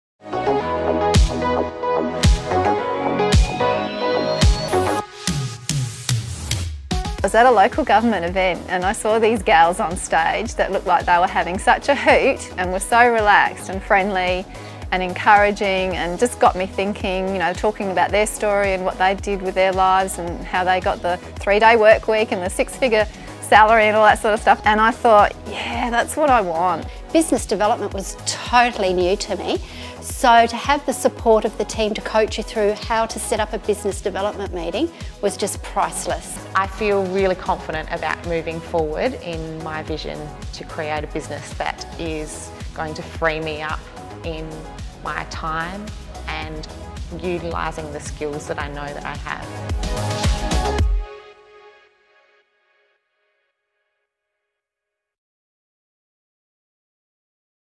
Lifestyle Business Builder 2020 - Coming Soon
Our 2020 LIFESTYLE BUSINESS BUILDER program is coming up SOON! What did last years students have to say about it?